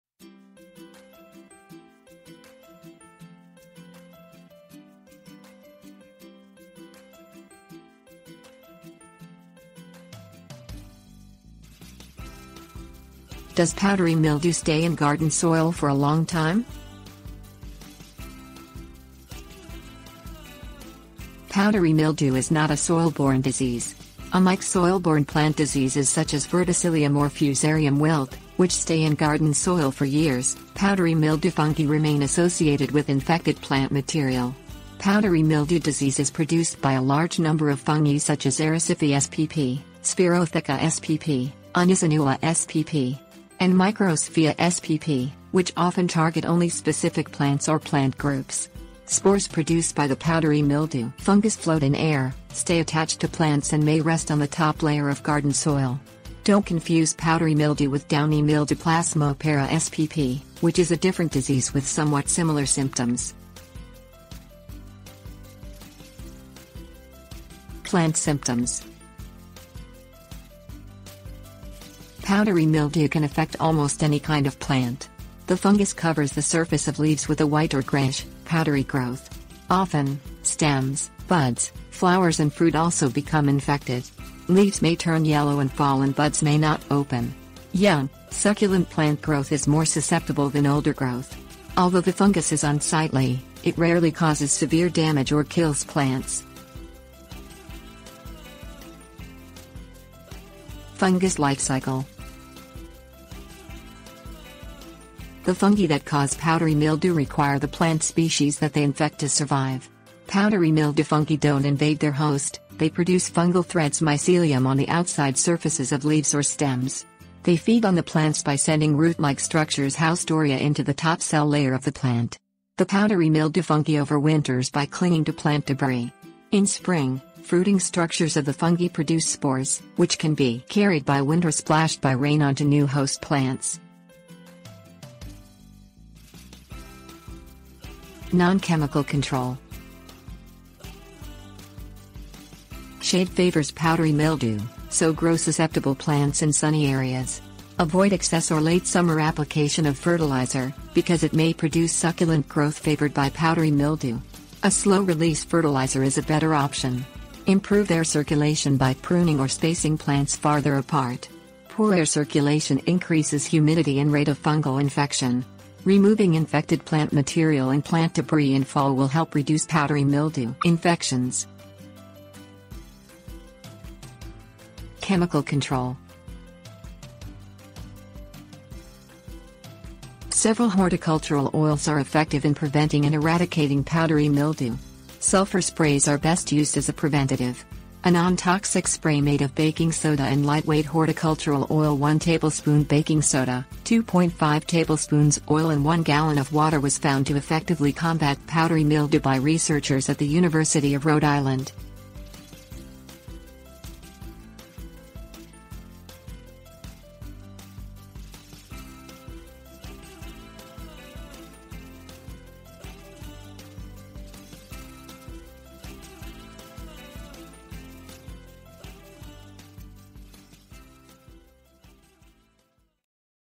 Does Powdery Mildew Stay in Garden Soil for a Long Time?
Does Powdery Mildew Stay in Garden Soil for a Long Time?. Powdery mildew is not a soilborne disease. Unlike soilborne plant diseases such as verticillium or fusarium wilt, which stay in garden soil for years, powdery mildew fungi remain associated with infected plant material. Powdery mildew disease is produced by a large number of fungi such as...

Table of contents Does Powdery Mildew Stay in Garden Soil for a Long Time?
Plant Symptoms 01:06
Fungus Lifecycle 01:39
Non-Chemical Control 02:17
Chemical Control 02:54